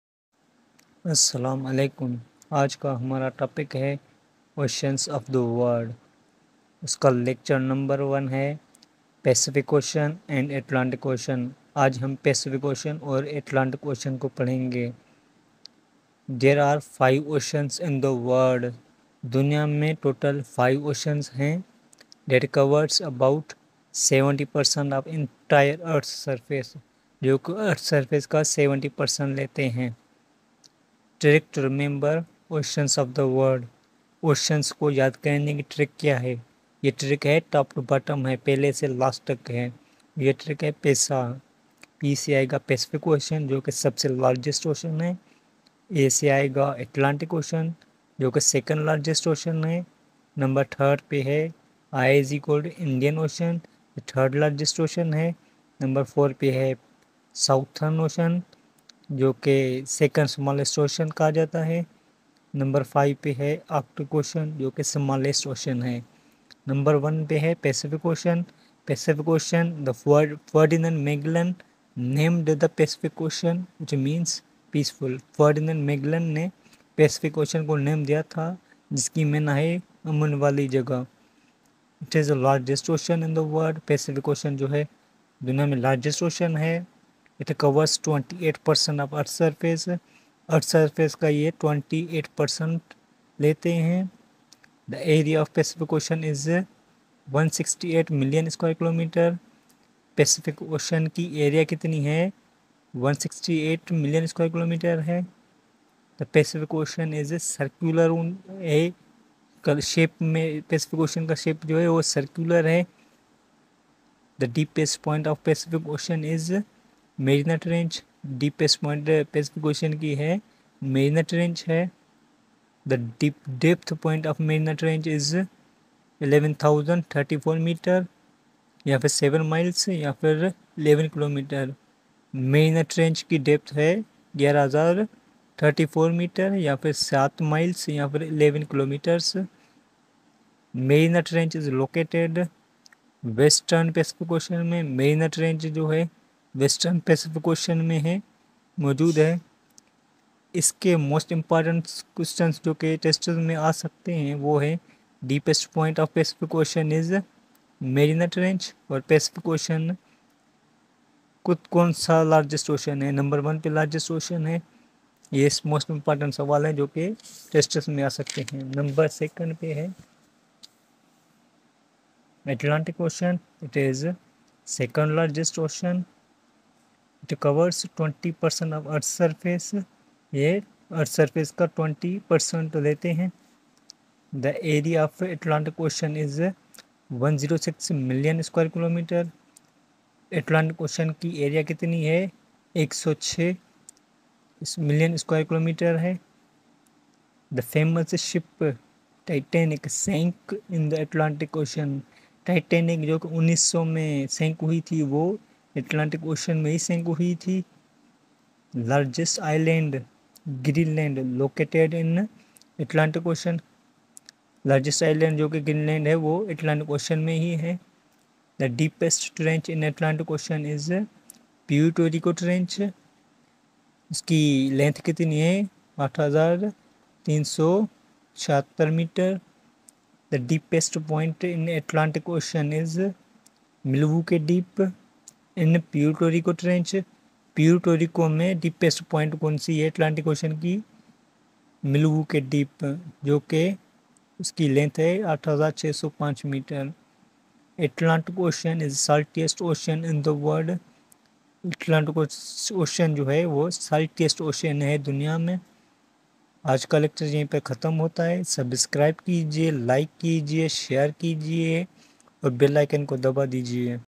Topic Oceans Of The World Lecture 01 Pasific and Atlantic Ocean
Topic Oceans Of The World Lecture 01 Pacific and Atlantic Ocean